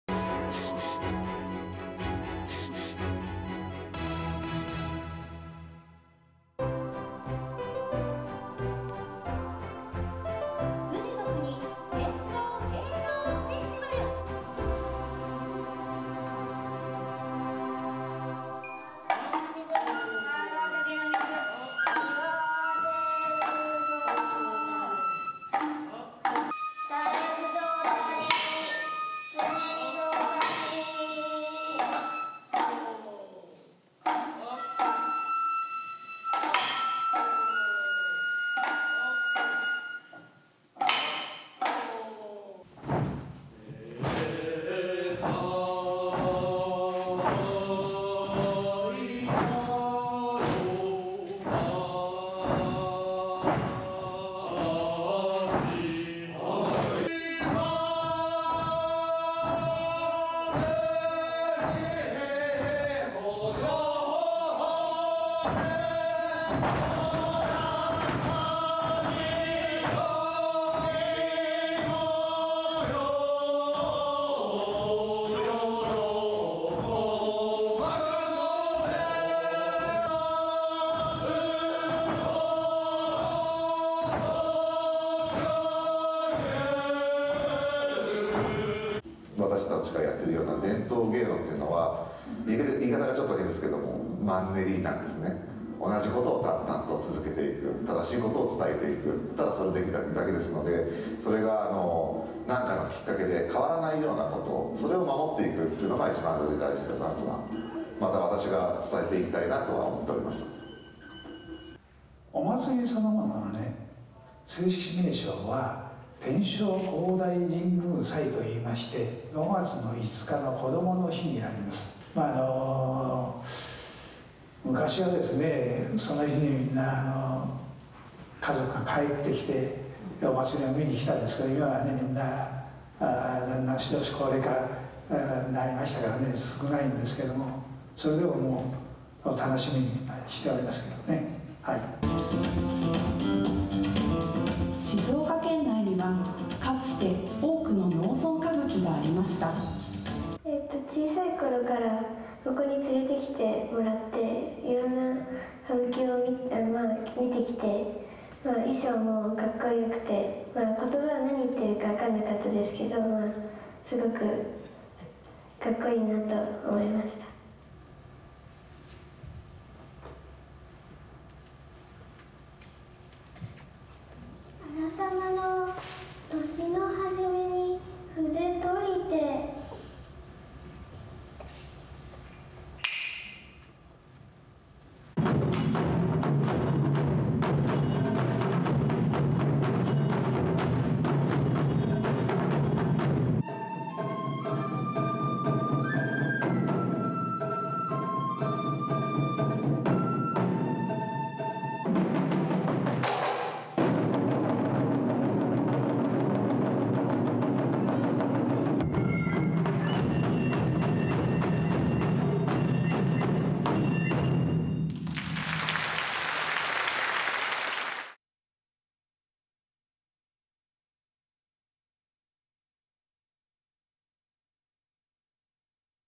静岡県文化プログラム（県域プログラム）ふじのくに伝統芸能フェスティバル
静岡県では、東京2020オリンピック・パラリンピックに向けて「地域とアートが共鳴する」をテーマに掲げ、文化プログラムを県内各地で展開しました。
５年間にわたる静岡県文化プログラムの活動についてはこちらをご覧ください。

◆静岡県文化プログラム
①東京2020 ＮＩＰＰＯＮフェスティバル共催プログラム　ふじのくに野外芸術フェスタ2021静岡